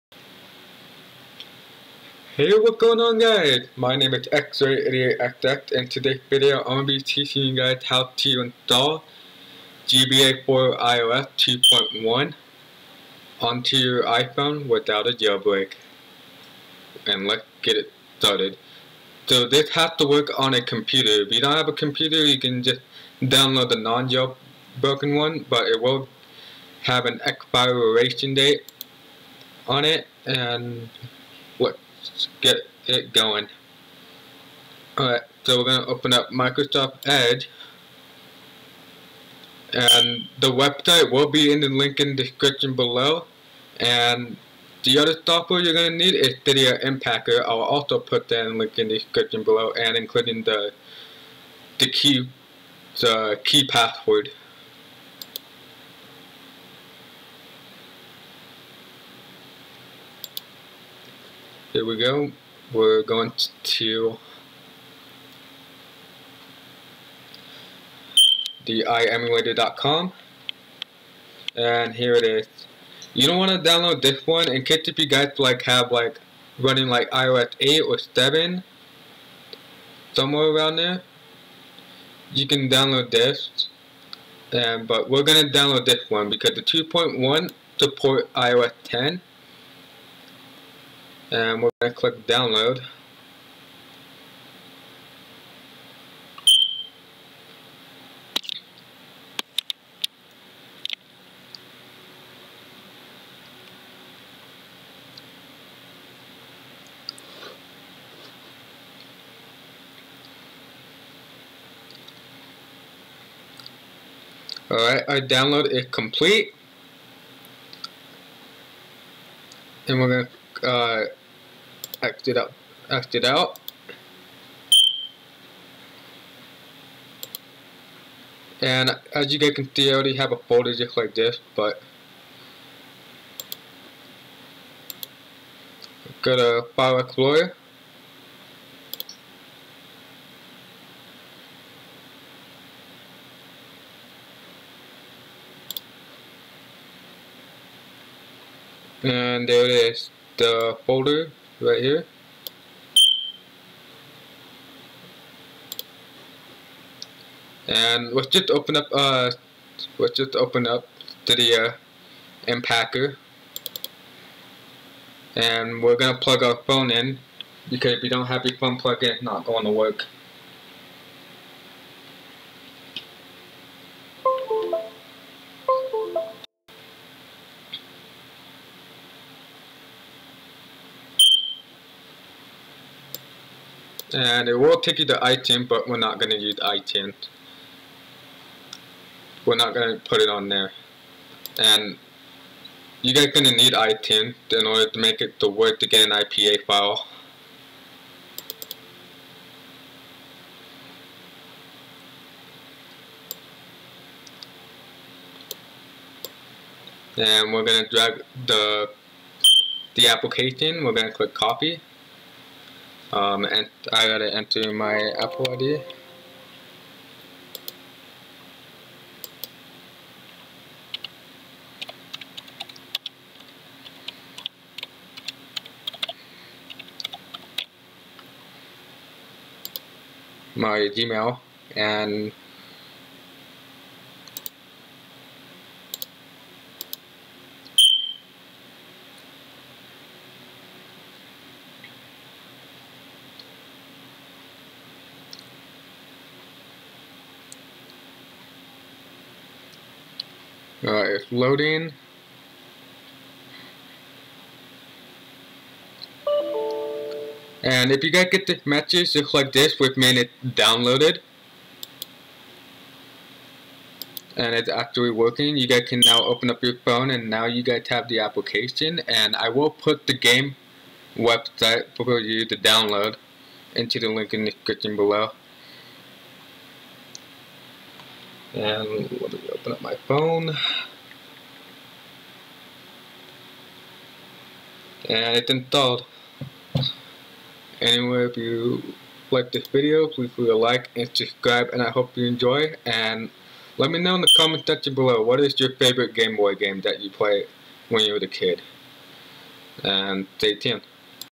how to install GBA4IOS on your iphone with ios 10 (computer only) no jailbreak and non jailbreak
(Note) to get the password you must go to the Apple website and sign into your account scroll down where it said generate app security click on generate and it give you the code Copy and paste on the cydia impactor this only work on a computer not on mobile